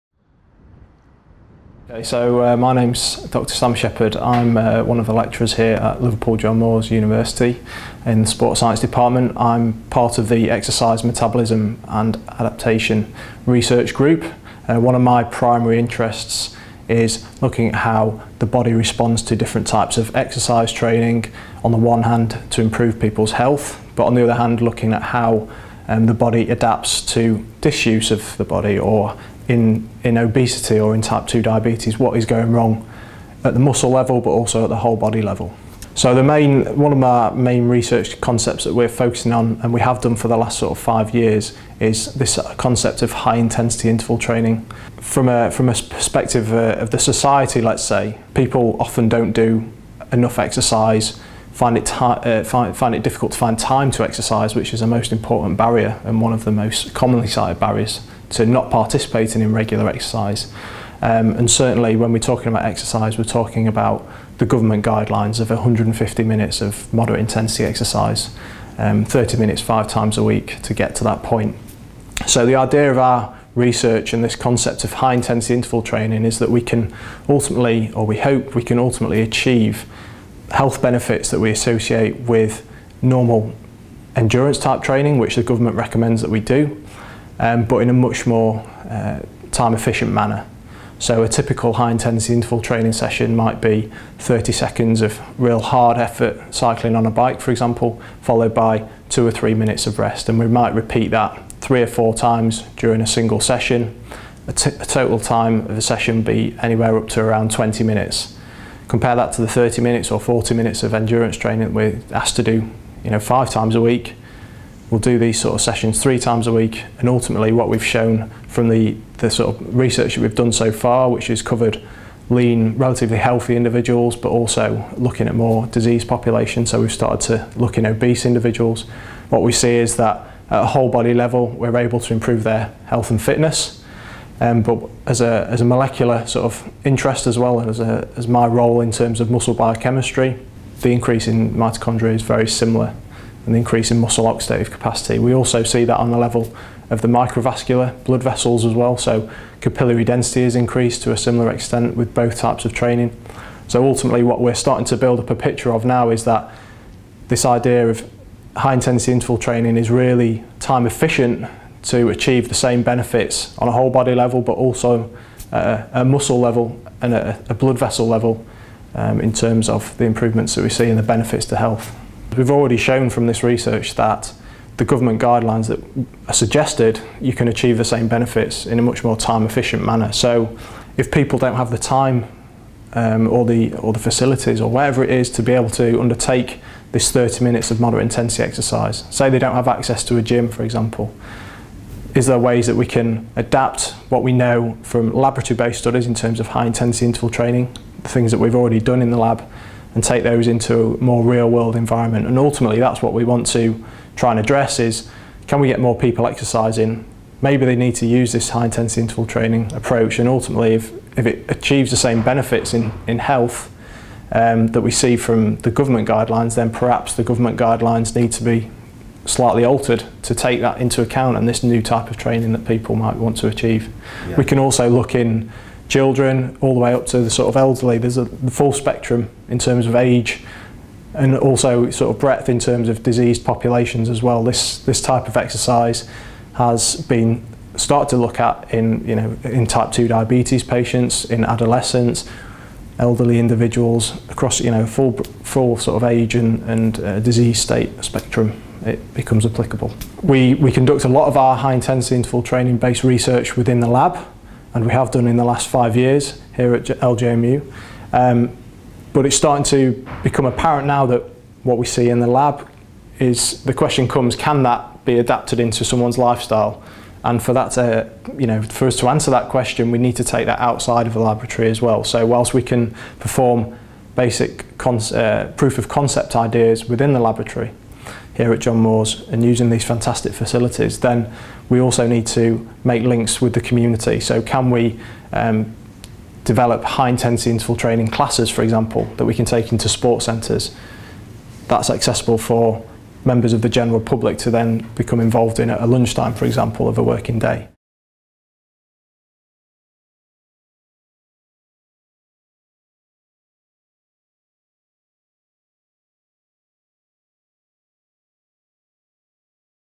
Exercise Metabolism and Adaptation Research Group: Dr Sam Shepherd
The Exercise Metabolism and Adaptation Research Group (EMARG) measure how scheduling of exercise, nutrition, ageing, injury and drug use affect the rate and the extent of muscle adaptation.